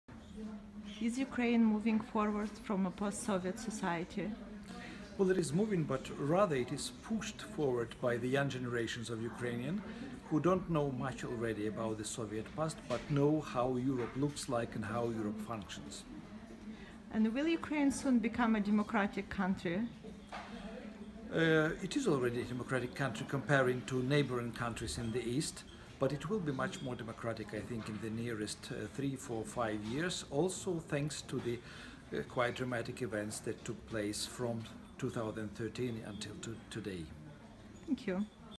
Andrei Kurkov
Andrei Kurkov, Ukraine novelist and independent thinker about Ukrainian democratic future